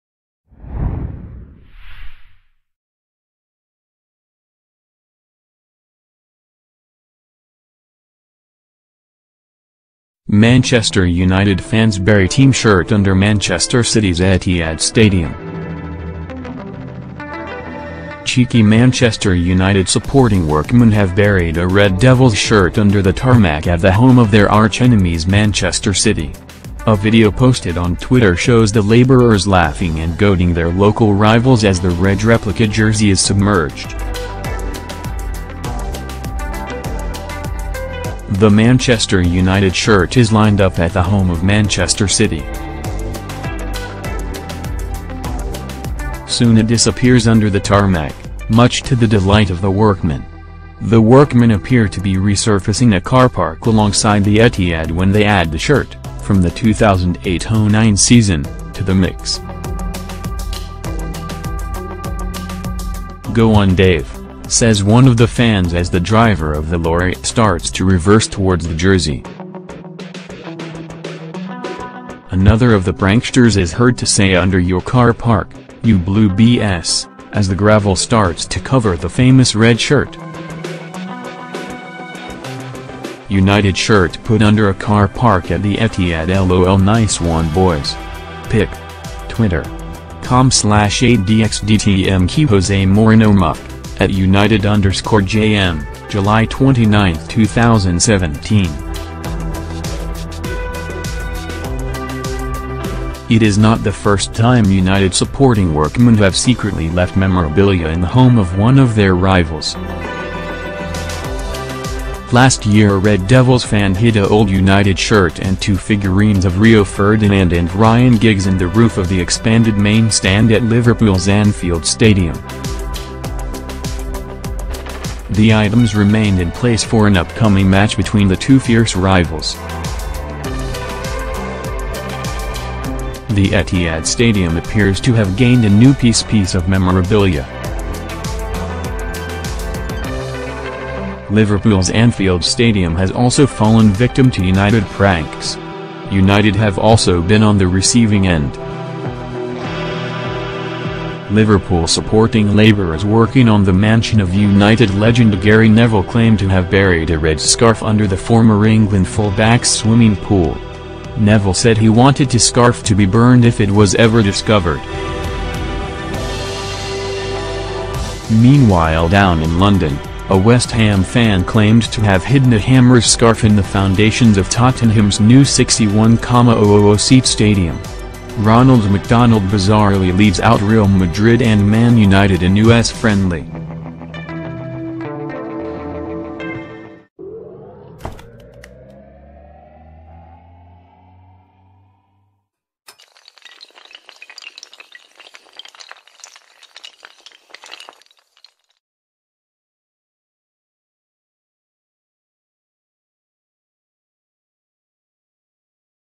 Manchester United fans bury team shirt under Manchester City’s Etihad Stadium | The Red Devils
Video posted on Twitter shows workmen laughing as they hide Red Devils jersey under Tarmac in car park at home of their arch rivals
We are The Red Devils